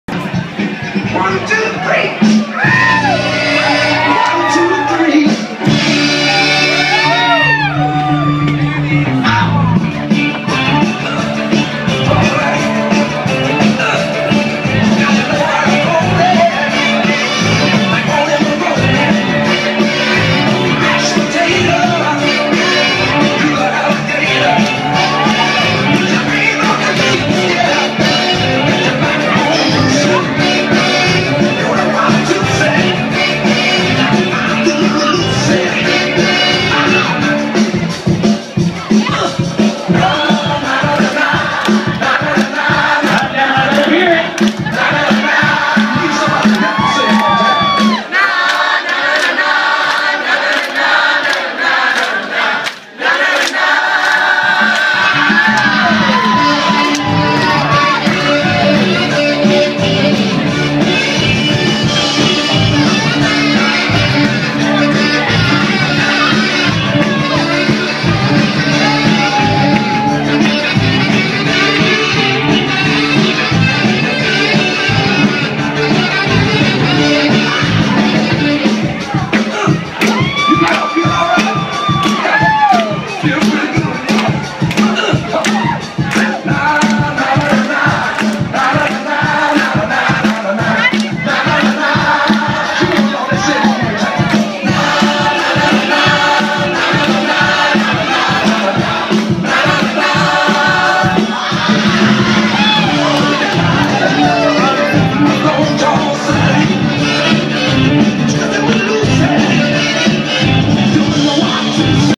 MISSING LINK'S SOUL NIGHT: The Freaky KiKi Dashiki Party 1/18/2013
Missing Link DJs Matt 'N' Adam bring the house down with one of the doapest cuts of the 20th century.

C///+\\\W